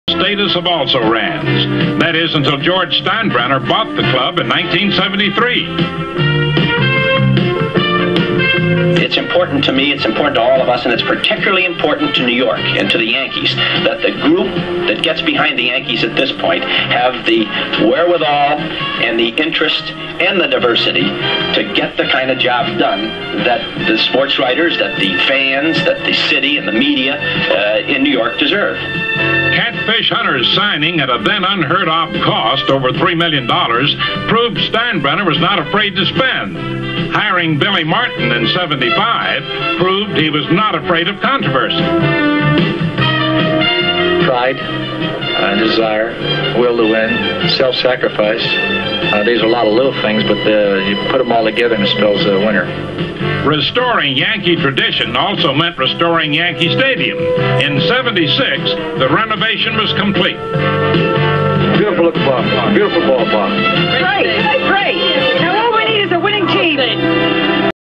New York Yankee Bobby Murcer Goes Between Monuments To Field Ball!
Great clip of New York Yankee Bobby Murcer going in-between the monuments on the field at Yankee Stadium to get baseball. Also, a very insightful interview clip with Yankee Owner George Steinbrenner!